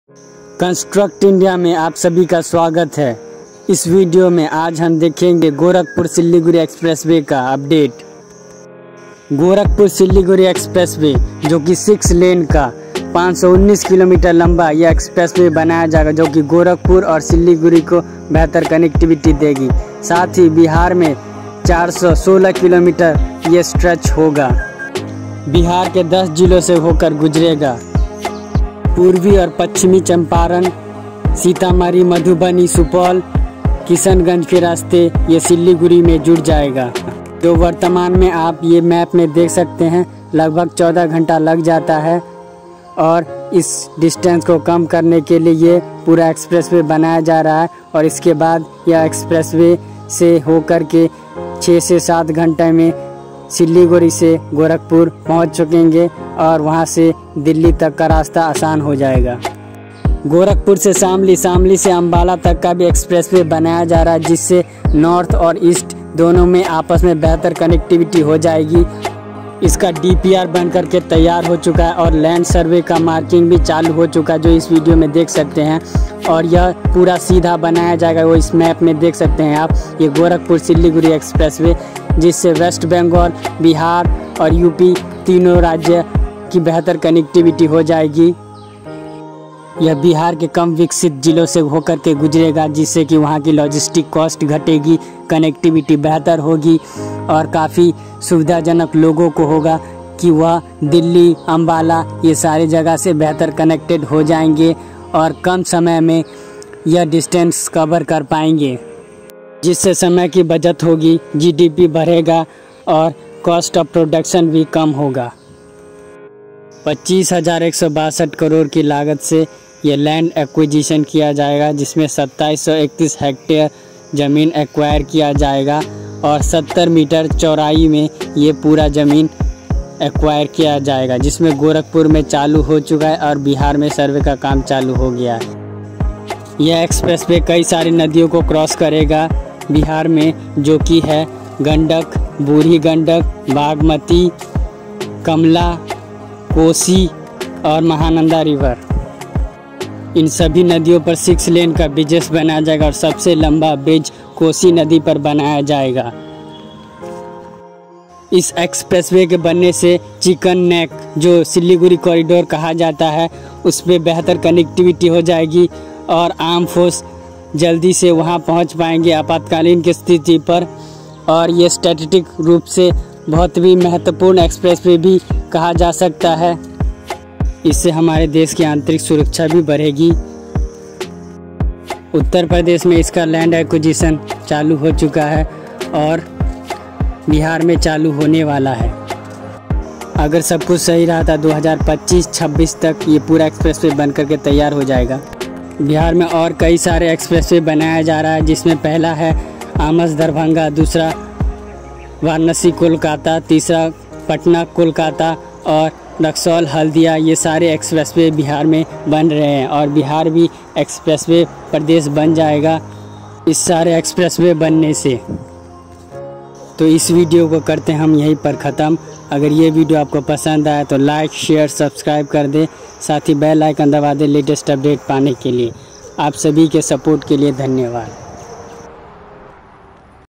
Ghorakpur siliguri expressway kaam chalu/गोरकपुर सिलिगुड़ी एक्सप्रेसवे काम चालू.
All video use above are free for commercial use without  atrubution.

In this video we inform the latest updates about ghorakpur siliguri expressway wich was 519 km long and have 416 km stress in bihar. This expressway passes through 10 district of bihar. The work of land aqusation under progress in up and survay in bihar after that land aqusation will start in bihar too.